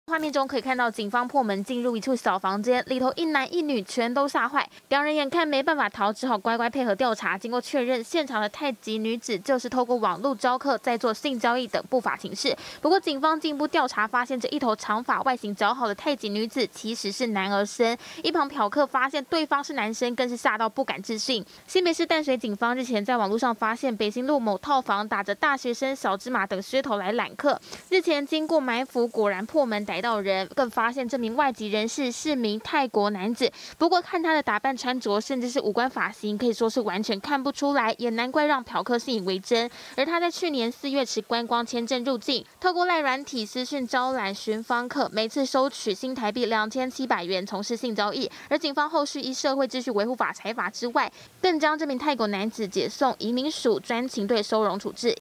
警搗破性交易場所 眼前女子竟是「男人」【最新快訊】
新北市淡水警方，日前經過埋伏，在一處套房發現有人在從事性交易等不法情事，經過破門搜查逮到兩人，更意外發現眼前外型亮眼的「女子」，竟然是男兒身，詳細情形，連線記者，潘玟諭。